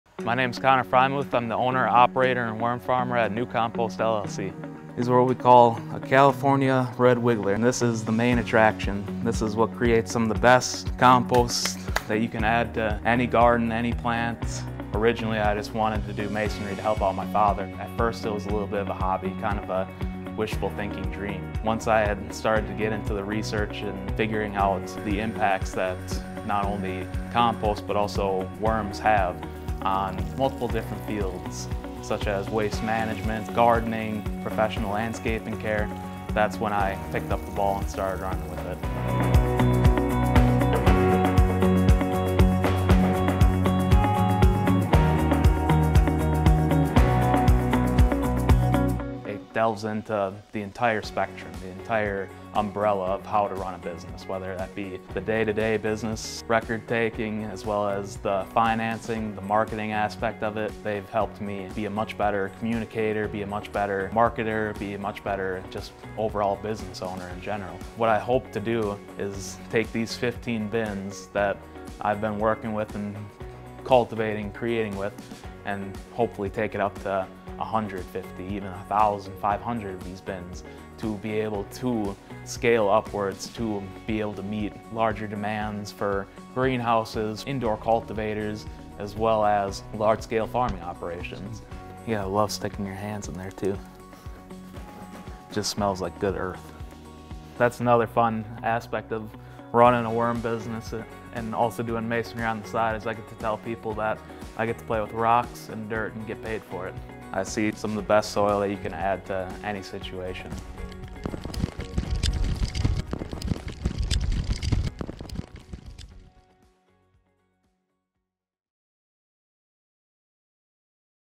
Got Worms?
Connor Freimuth started out working for his father's masonry business.  But a couple of years ago, he found a new calling - Worm Farming, and started his own business, New Compost LLC.  In 2023, Connor enrolled at Fox Valley Technical College for the E-Seed Entrepreneurship class to help him learn how to better run his business.